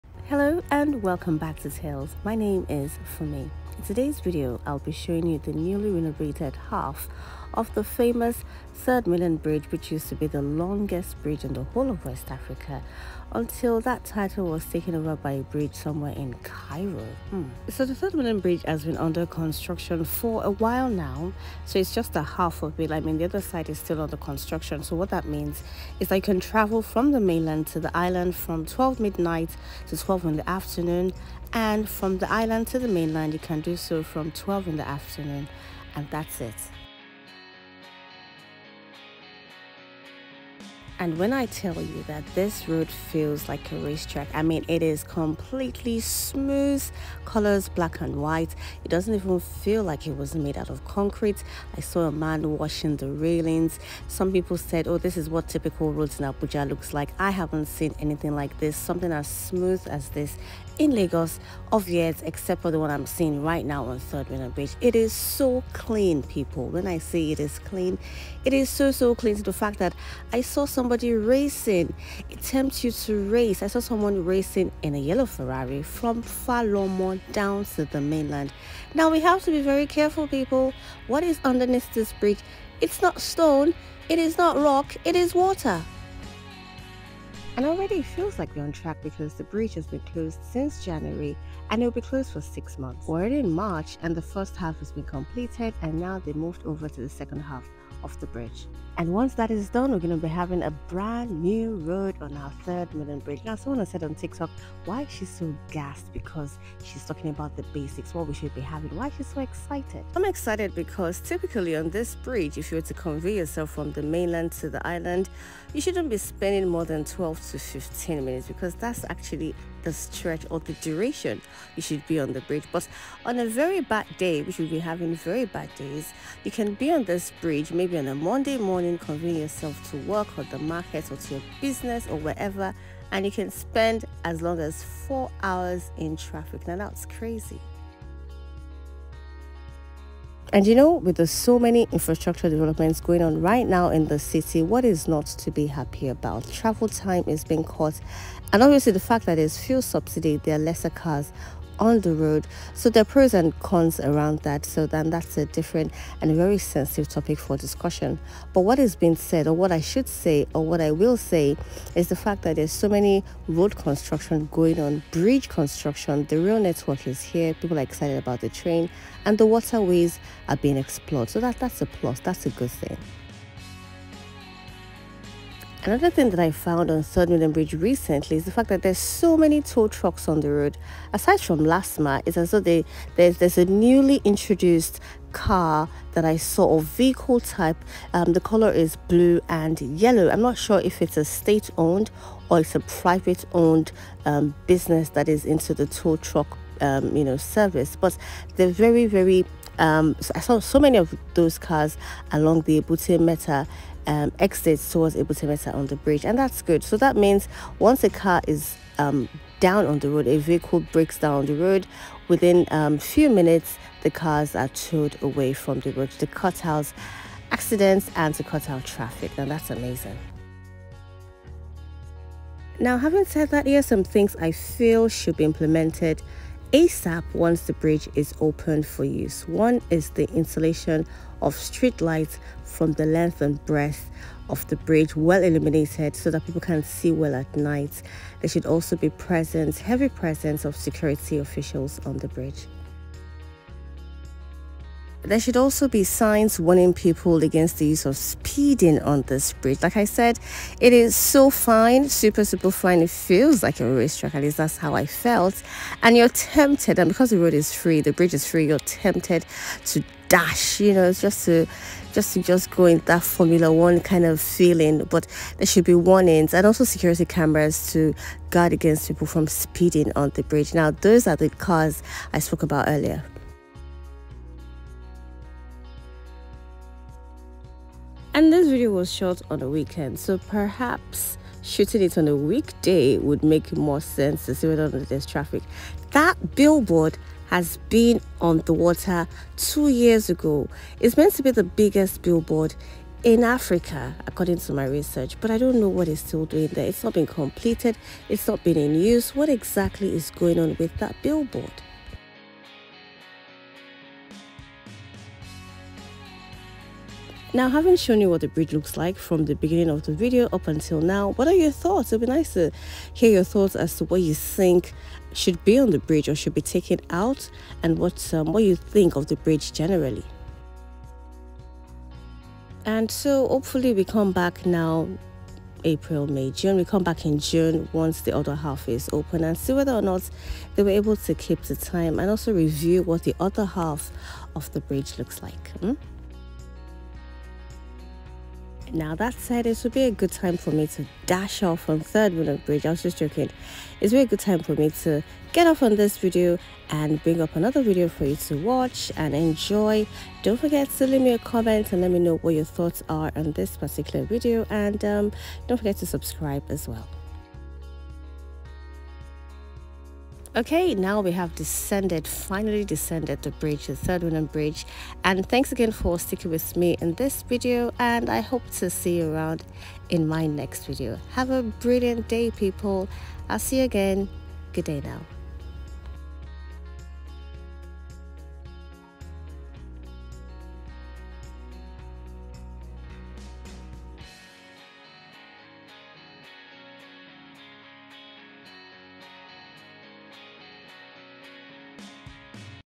Is the newly renovated Third Mainland Bridge in Lagos, Nigeria now a Race Track?
Discover the breathtaking transformation of Lagos' iconic Third Mainland Bridge in 2024 into a marvel of engineering resembling a racetrack! Explore the incredible renovations and witness firsthand the unparalleled blend of innovation and beauty. Prepare to be amazed by this architectural wonder, a testament to human ingenuity and progress in infrastructure. Don't miss out on experiencing this unparalleled feat of modern engineering in one of Africa's most dynamic cities.